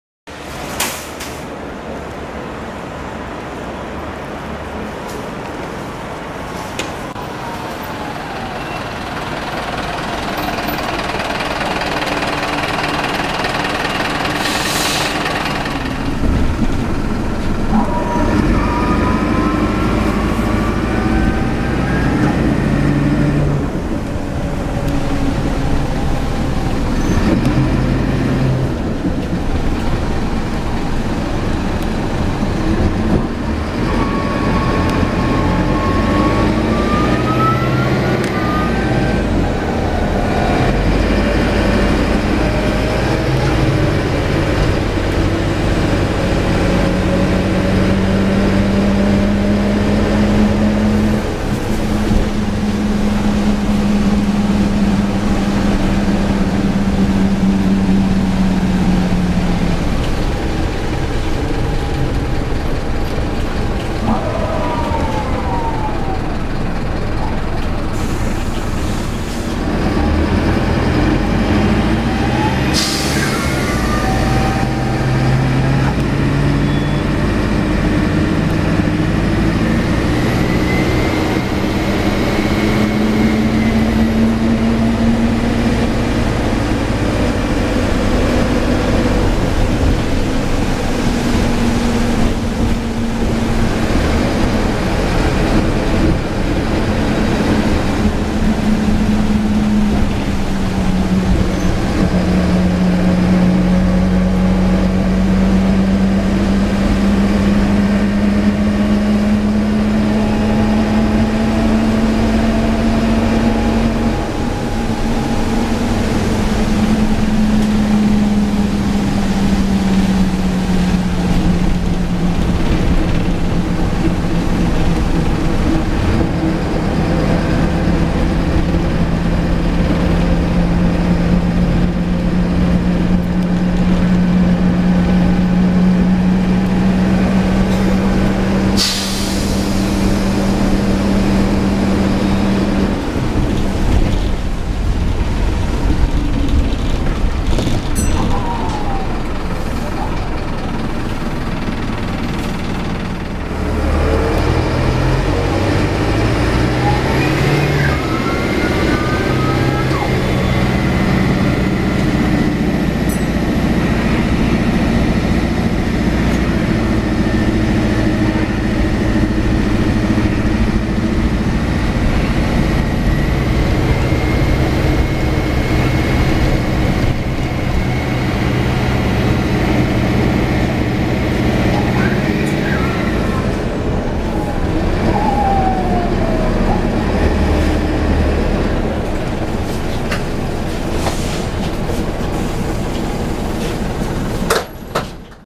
Optare Solo KX51CSF Stagecoach Cumbria & North West Lancashire
A ride on Optare Solo KX51CSF from Kendal to Oxenholme.